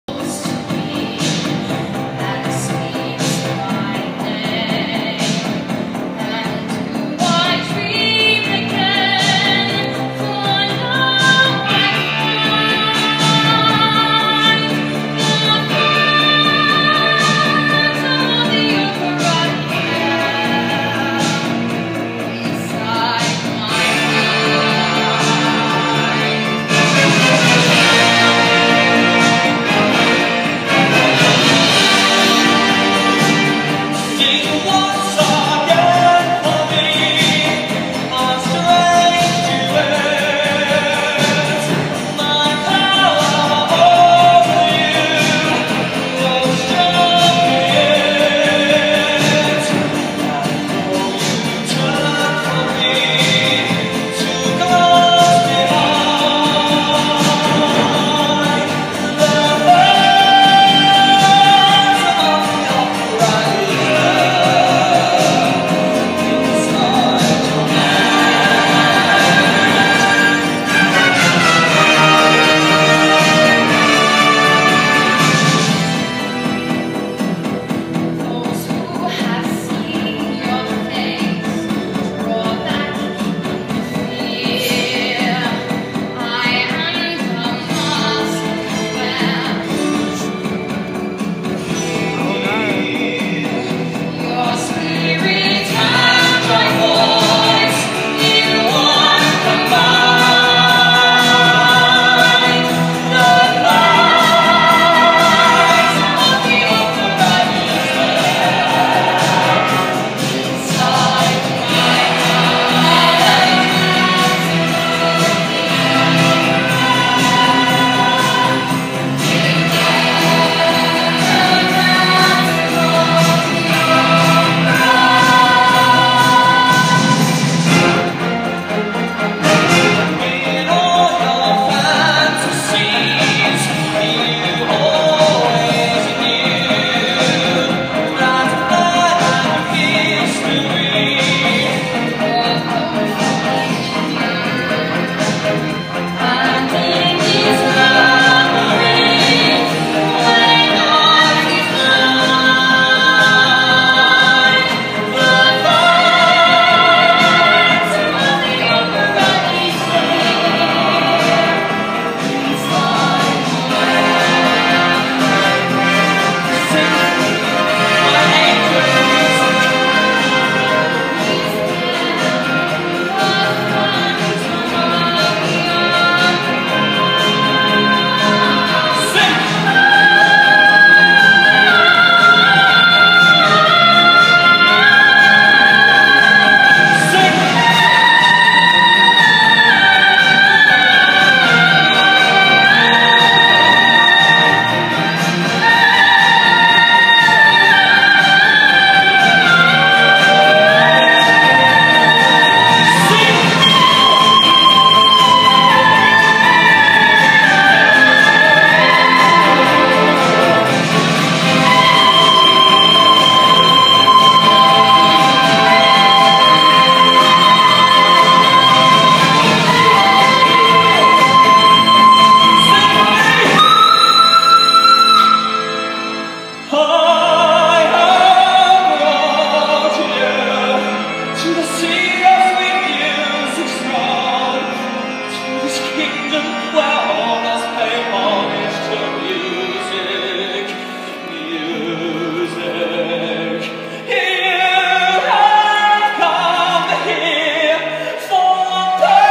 Katie Walder singing
Joby Morris and my daughter Katie Walder hitting the high notes Nov 2014